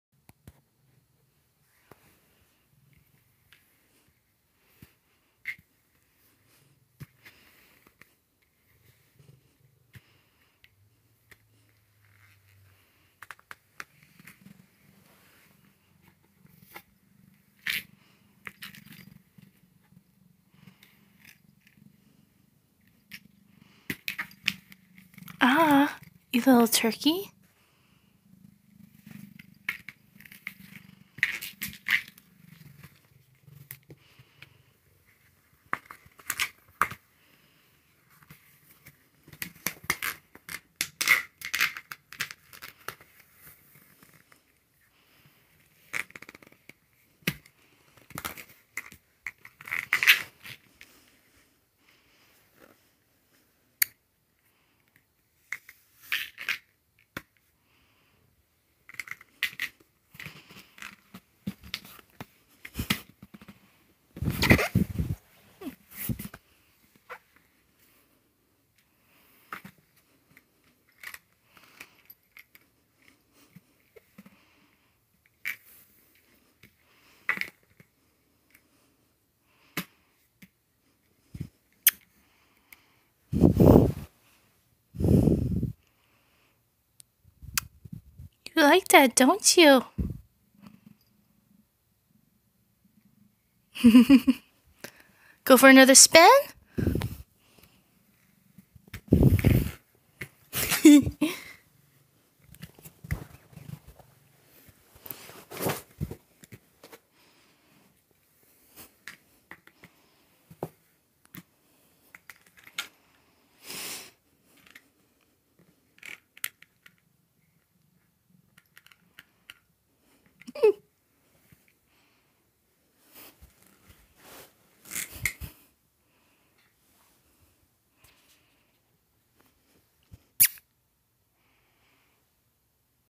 Kitty Goes for a Ride in Her Ball
See title. Ball is abandoned street lamp cover. Not sold in stores. Unbridled joy for Lillypad.